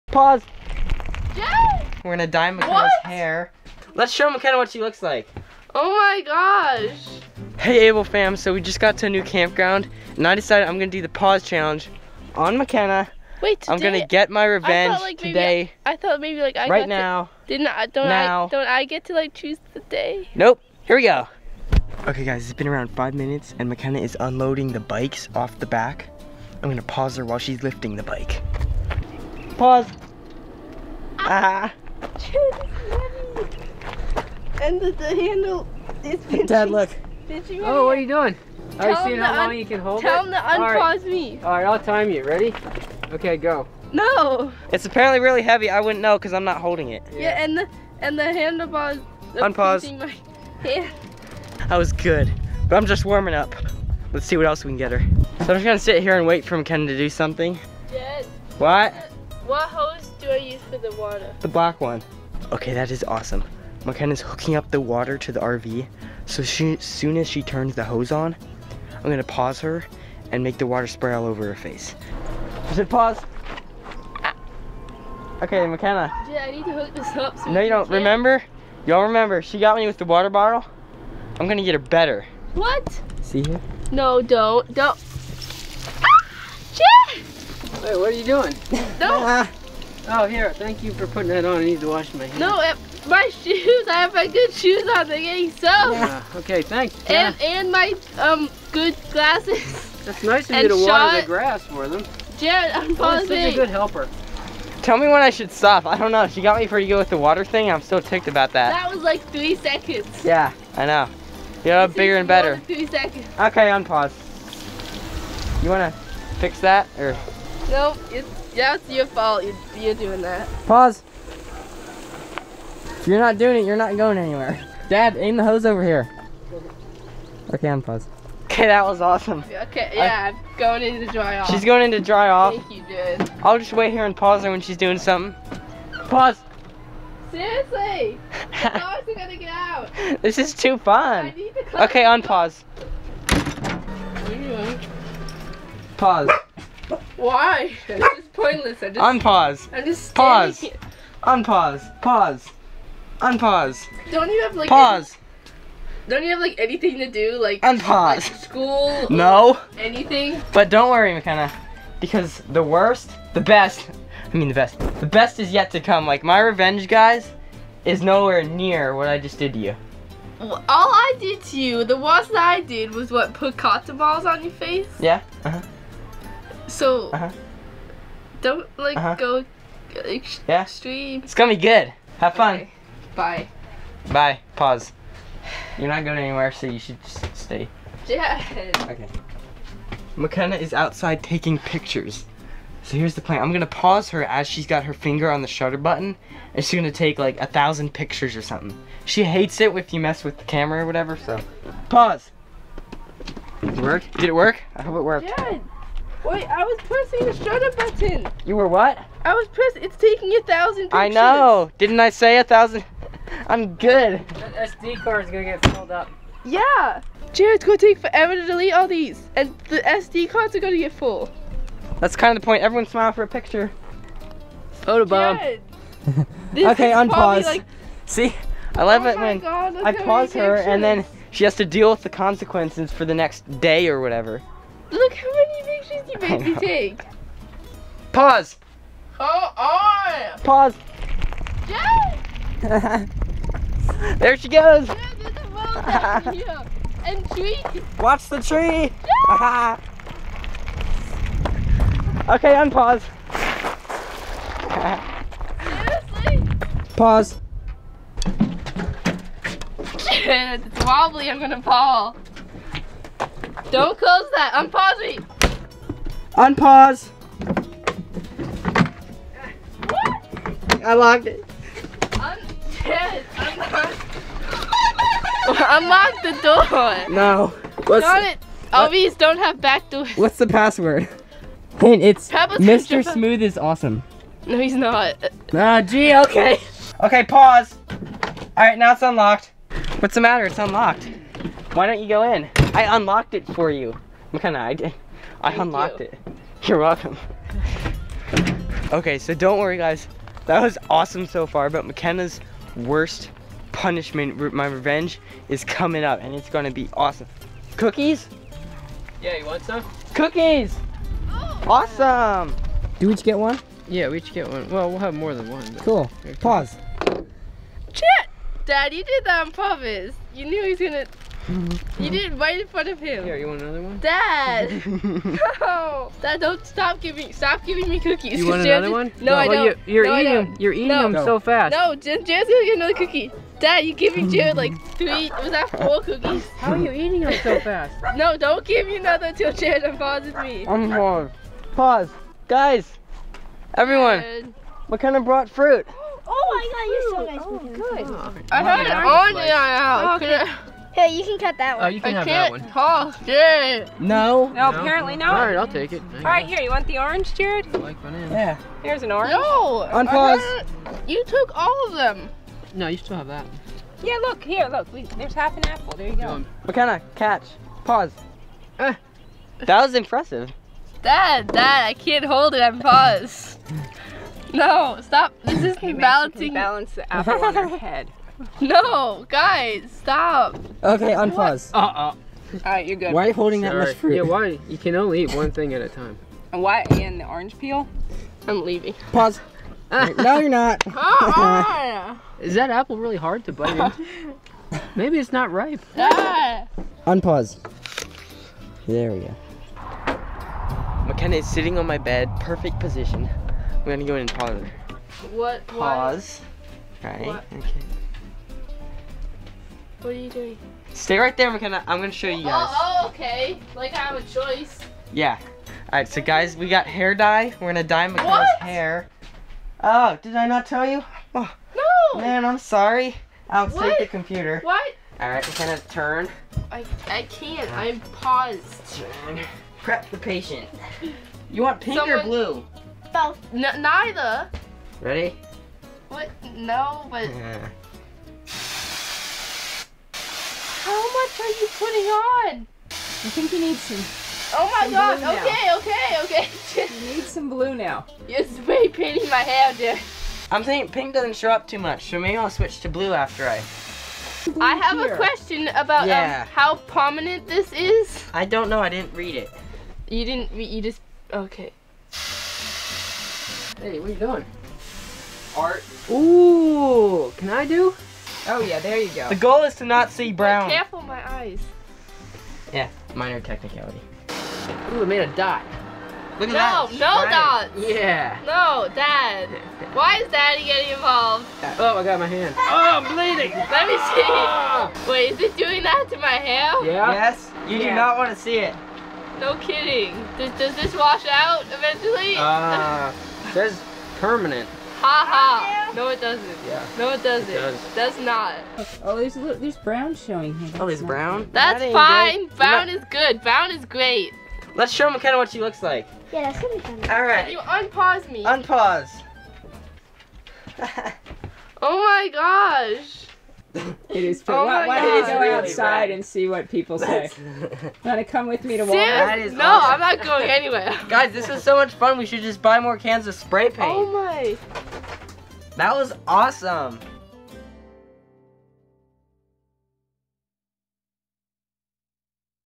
BOY GIRL TWIN PAUSE CHALLENGE - DYEING MY TWIN SISTERS HAIR
Jarod got revenge on his twin sister in our second pause challenge; he dyed her hair crazy colors while she was paused!